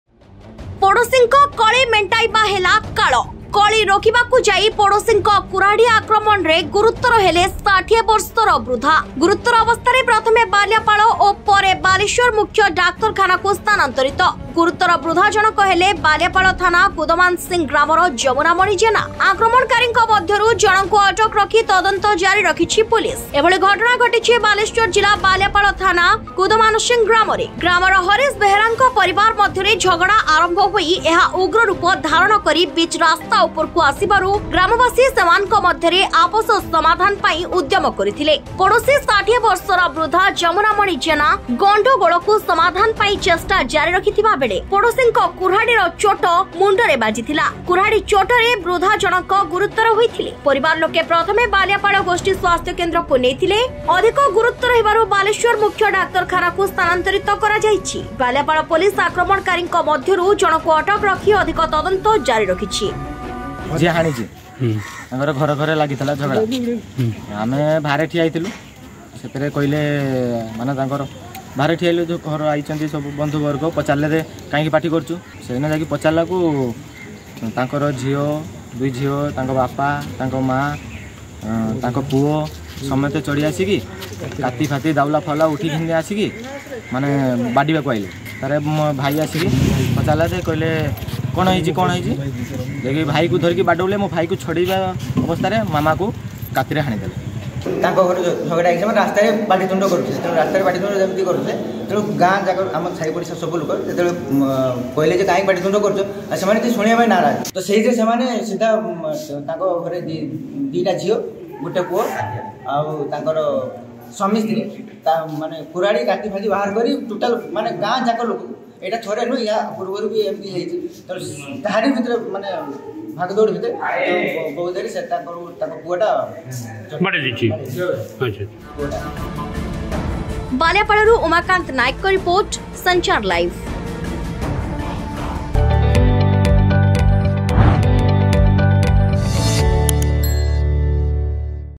ପୋଡ଼ଶୀଙ୍କ କଳି ମେଂଟେଇବାକୁ ଯାଇ ଗୁରୁତର ହେଲେ 60ବର୍ଷର ବୃଦ୍ଧା || Sanchar Live
ପୋଡ଼ଶୀଙ୍କ କଳି ମେଂଟେଇବାକୁ ଯାଇ ଗୁରୁତର ହେଲେ 60ବର୍ଷର ବୃଦ୍ଧା